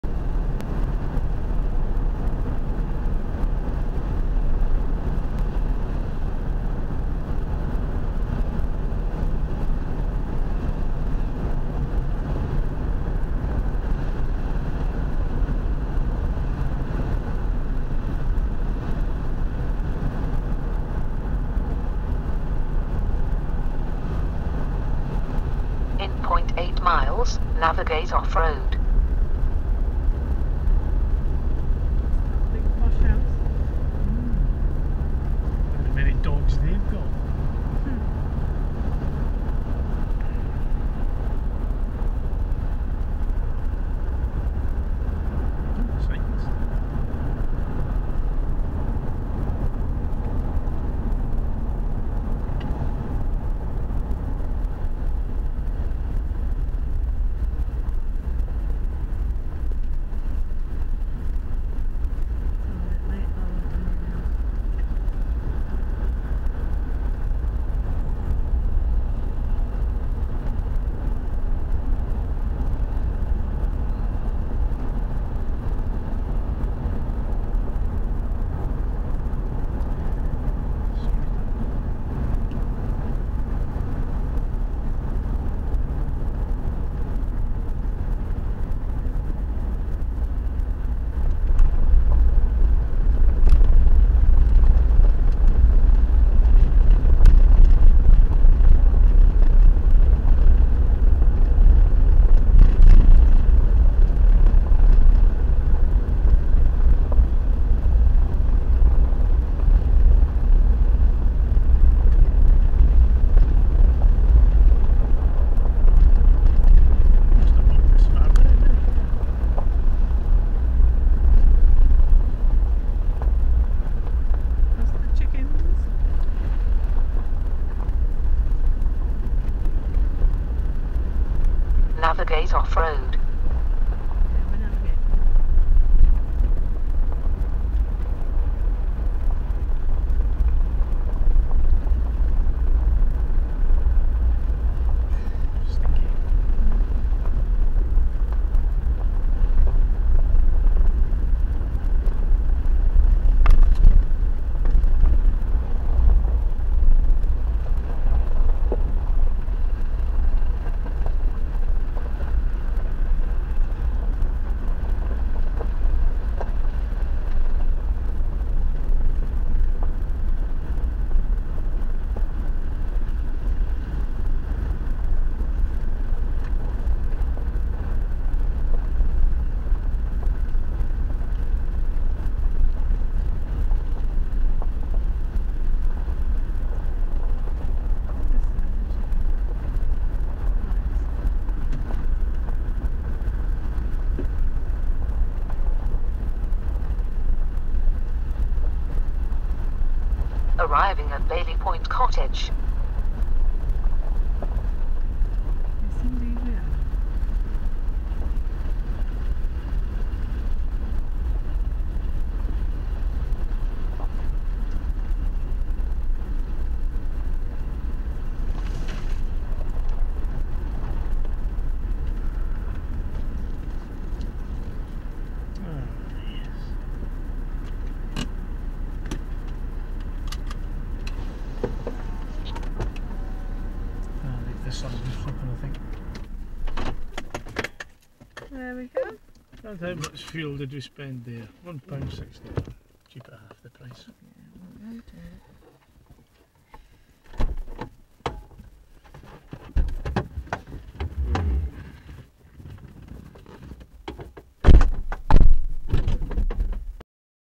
Drive to Bailey Point Cottage
This is the drive from just of the main road to our holiday cottage.